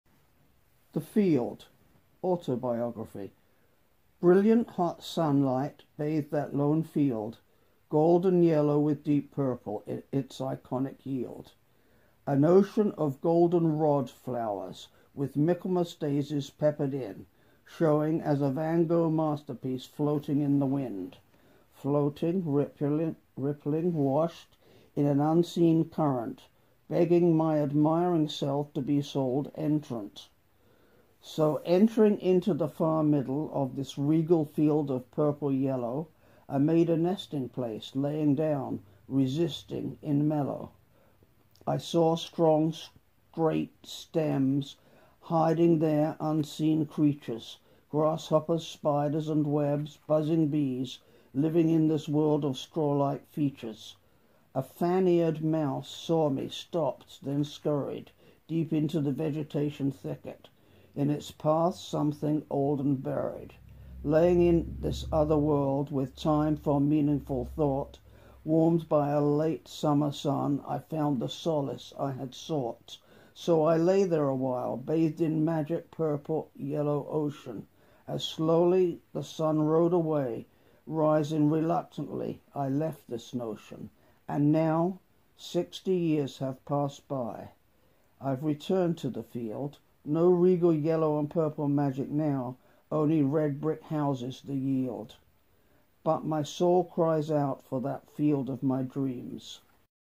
The Field | Michaelmas Daisies - Poets & Painters 2021
The featured poetry (audio) is “The Field” by L. Taylor. The full text is below.

The painting response is “Michaelmas Daisies” by A. Wildman.
Acrylic on canvas. 16 x 12 in.
“Sixty years later, Lesley Taylor returns to a field where she once lay “bathed in a magic purple-yellow ocean” and saw Michaelmas daisies “floating in the wind”. The Michaelmas Daisy provides nectar and pollen for bees – which we need to survive. The “solace” Lesley found then and her “field of dreams” are gone. "Michaelmas Daisies" captures the “magic”--the innocence, simplicity, and happiness of Lesley’s long August afternoon lying in the field.”

The Field

Brilliant hot sun, bathed that lone field.
Golden yellow with deep purple
Its Iconic yield.

An ocean of goldenrod flowers
With Michaelmas daisies peppered in,
Showing as a Van Gogh masterpiece,
Floating in the wind.

Floating, rippling, washed
In an unseen current;
Begging my admiring self
To be sole entrant.

So entering into the far middle
Of this regal field of purple- yellow
I made a nesting spot,
Laying down, resting in a mellow.

I saw strong, straight stems
Hiding there unseen creatures,
Grasshoppers, spiders and webs, buzzing bees
Living in this world of straw-like features

A fan-eared mouse, saw me, stopped then scurried
Deep into the vegetation thicket;
In its path something old and buried.

Laying in this other world
With time for meaningless thought,
Warmed by a late summer sun
I found the solace I had sought.

So I lay there awhile
Bathed in a magic purple-yellow ocean
As slowly the sun rode away.
Rising reluctantly I left this notion.

And now as 60 years have passed by
I’ve returned to the field.
No regal yellow and purple magic now,
Only red brick houses the yield.
But my soul cries out for that field of my dreams.